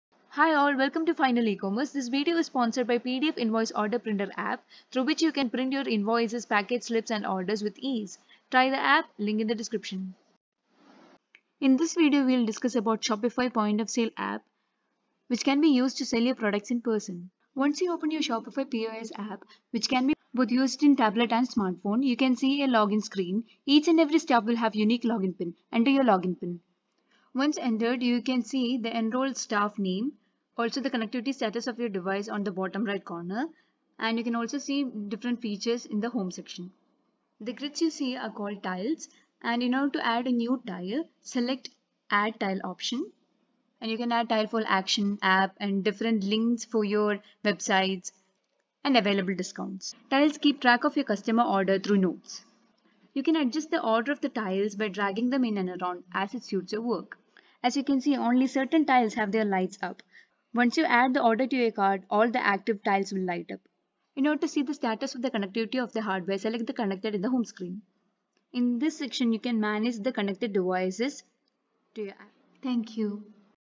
Shopify POS demo | pointofsale tutorial
Shopify POS system permits to sell your product or service in person, also it is a useful tool to capture customer's information and turn them into loyal brand advocates.
This video aim to familiarize the key features of Shopify POS app ,
to provide an understanding about tiles and their function, store workflow and staff access.